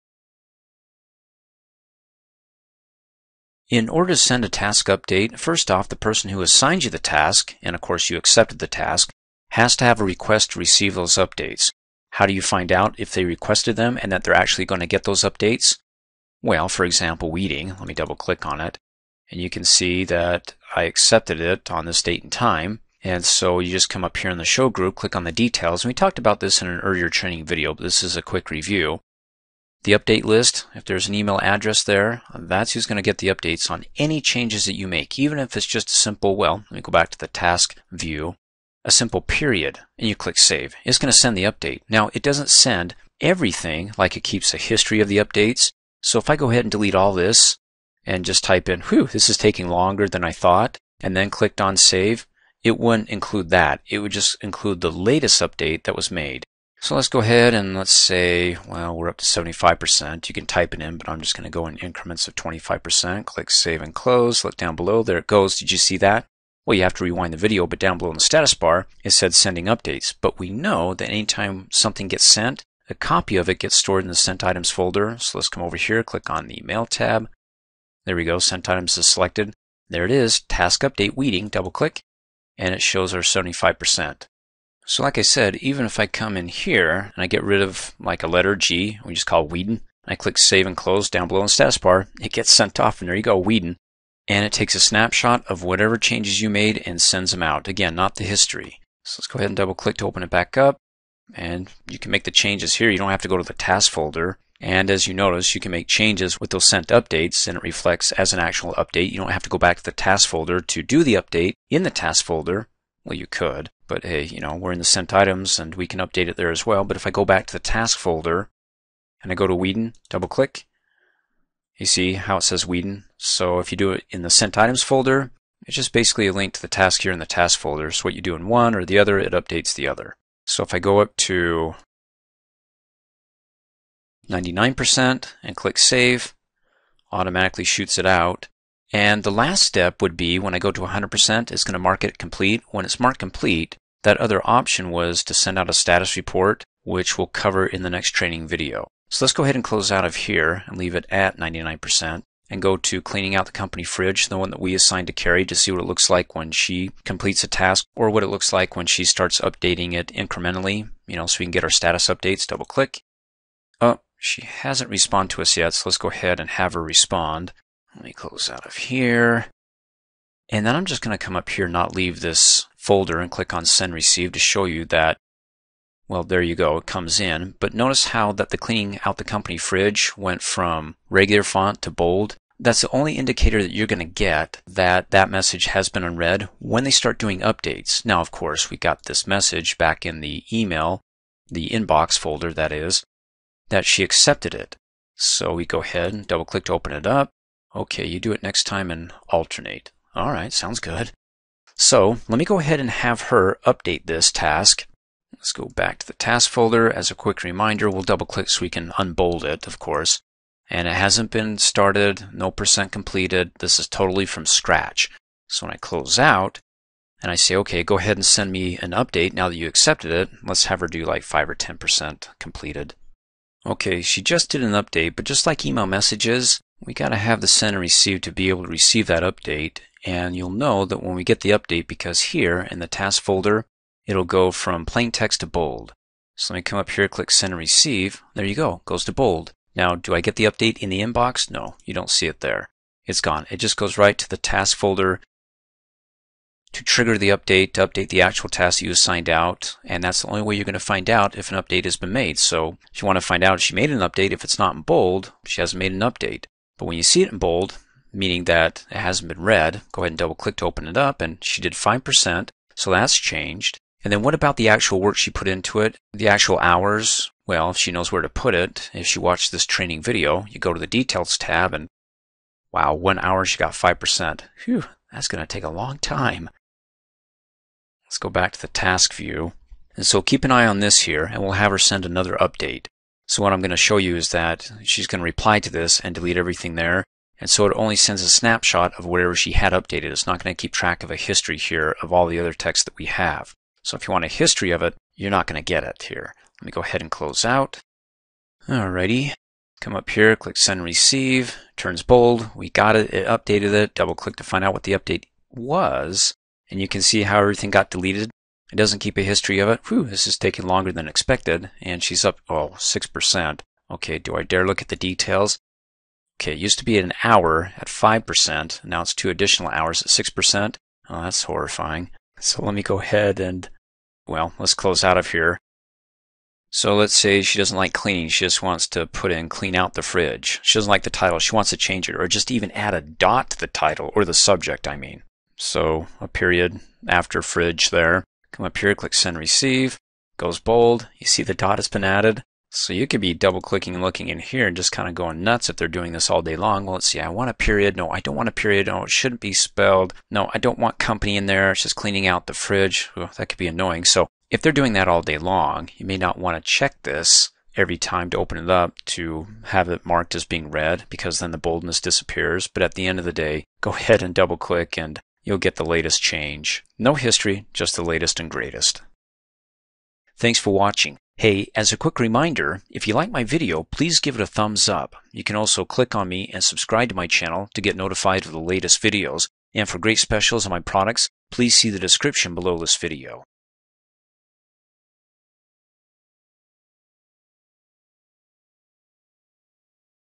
Microsoft Outlook 2016: Sending Task Updates
Microsoft Outlook 2016 training video on how to send Task Updates, or any updates you make to a Task including percent Complete, Due Date and Notes through email.